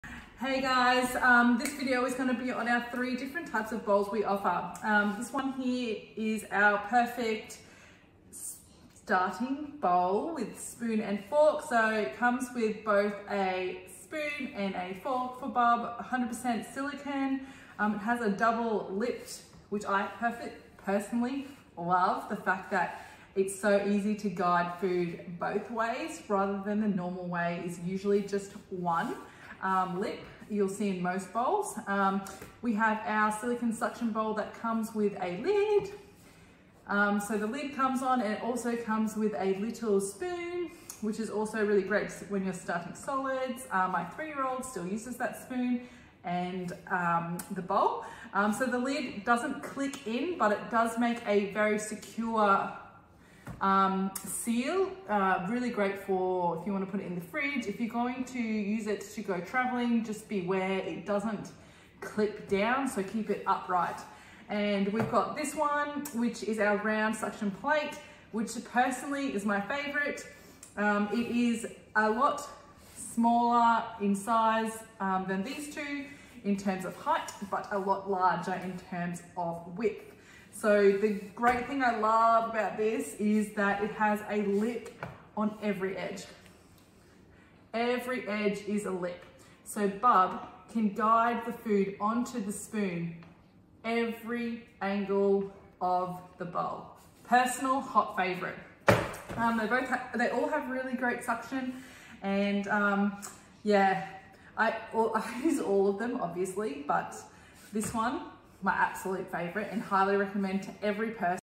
Which Bowl Should I Choose?
This video details the three bowls we currently offer:

- Silicone Suction bowl with spoon

- Perfect starting bowl with spoon and fork

- Round Suction Plate (included as is it flexible and can act as a bowl and a plate!)